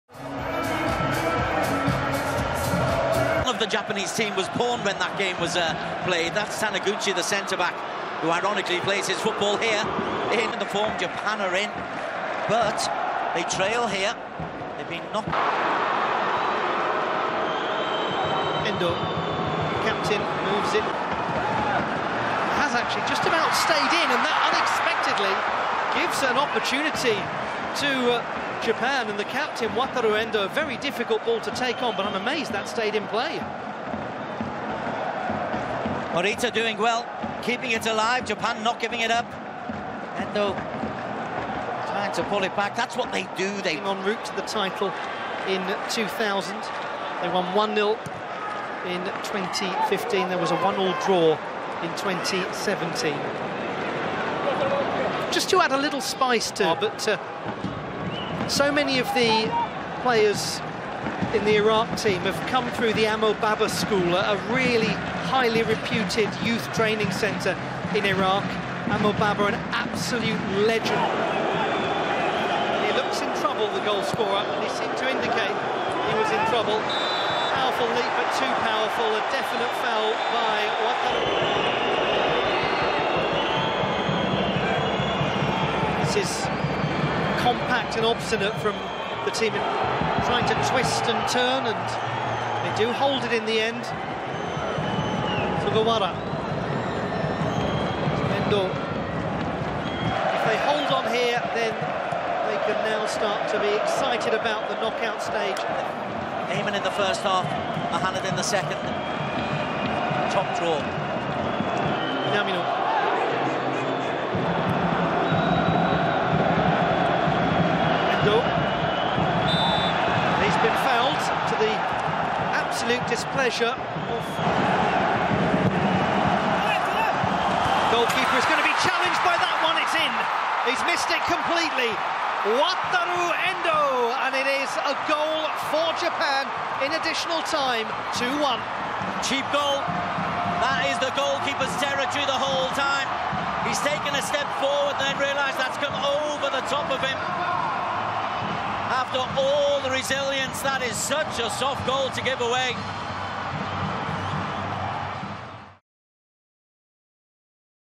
日本がイラクに敗れた試合で遠藤航がゴールを決めた。 (2024/01/19)
✅ フットボールユニフォーム 2023/24 を低価格で！
⏺ コード「HN20」を使用すると 20% オフになります